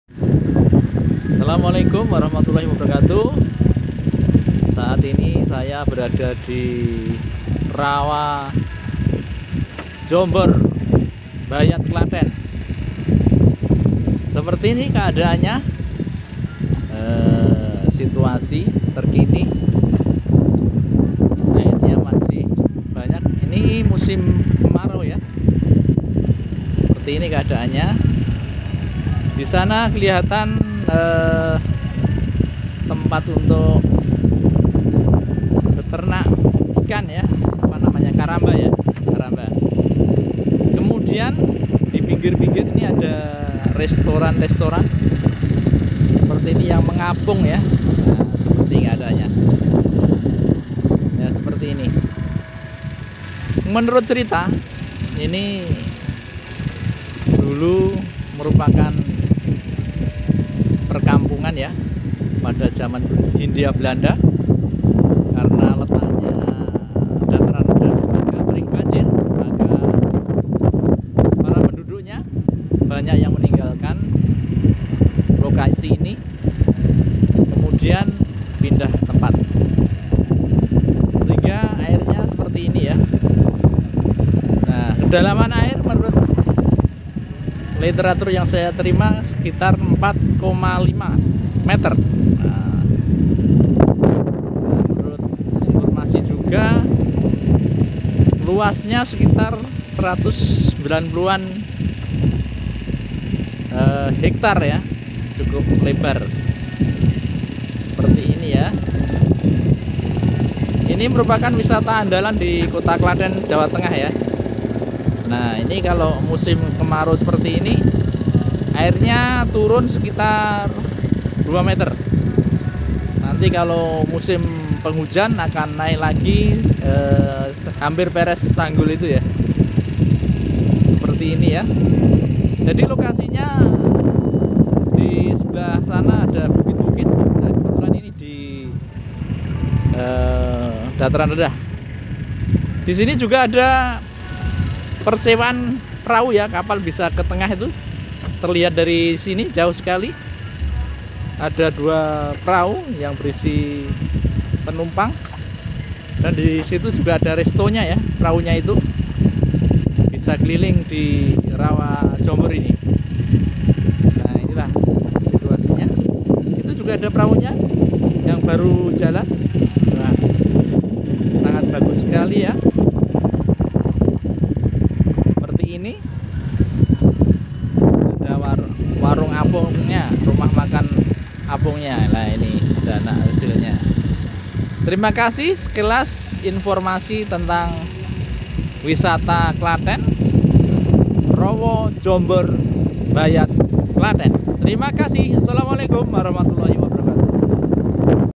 Rowo Jombor Klaten terbaru
Rowo Jombor Klaten terkini merupakan  tempat wisata di kabupaten Klaten Jawa tengah.
Wisata Rowo Jombor juga menyediakan menu menu makanan di seputar rawa. Selain itu juga naik perahu di tengah Rowo Jombor.